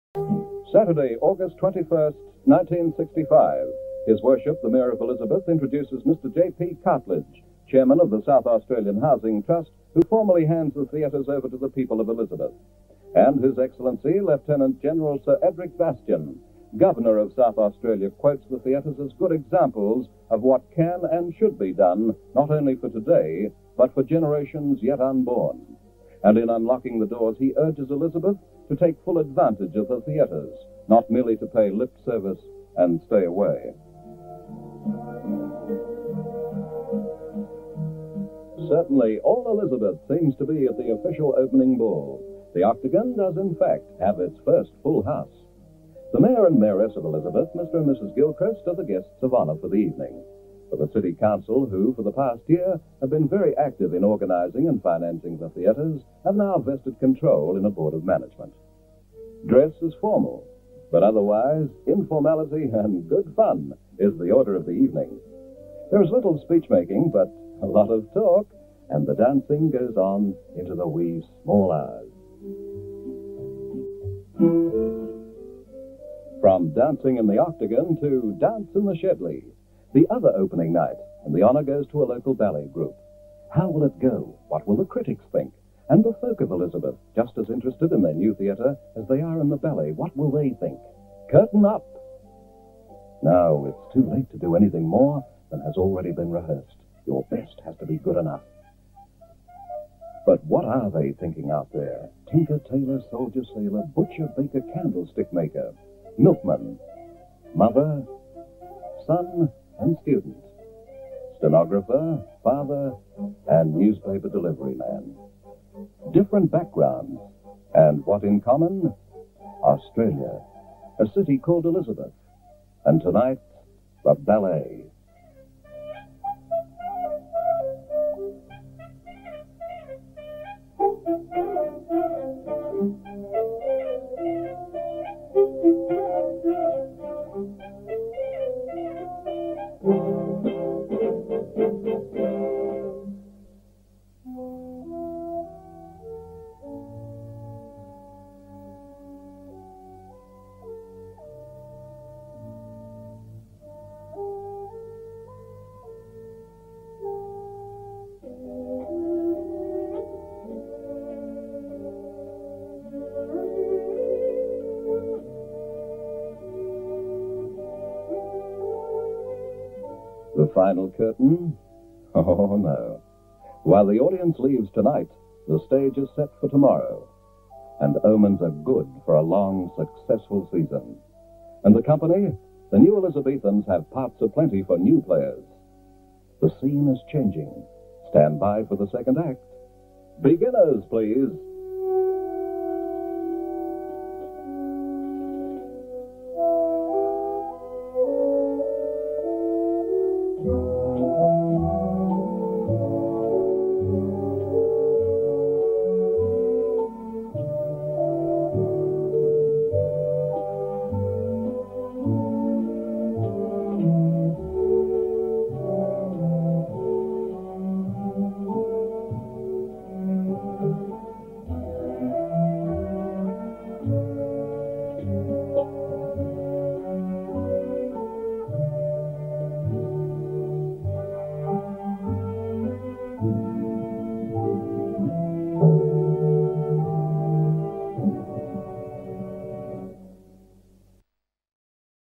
Elizabeth Civic Theatres: Opening Night 21st August 1965
The Elizabeth Civic Theatres were officially opened on 21st August, 1965 by the Governor of South Australia, Sir Eric Bastyan.
They were designed by Geoffrey Shedley, chief Architect of the South Australian Housing Trust, and built by the Elizabeth City Council at a cost of approximately half a million dollars.

The final design resulted in two separate units, an intimate theatre seating 412 named The Shedley after its designer and a stadium type theatre named The Octagon with a seating capacity of 1422.
The Octagon was designed to cater for any type of entertainment from stage shows, balls and cabarets to indoor sports, exhibitions, cinema events, boxing and promotional activities. Its popularity over the year has been marked by the addition of events such as musical concerts, eight-ball championships, darts championships, wrestling, Bingo and community activities such as large presentations, public meetings and baby shows to mention a few.

The Shedley had been home to several local groups such as the Elizabeth Repertory Company, the Northern Light Theatre Company, Mirrabooka and recently the S.A. Stage Company. It has also been the venue for productions by visiting groups and theatrical companies such as the Australian Dance Theatre, the Australian Elizabethan Theatre Trust and the South Australian Theatre Company.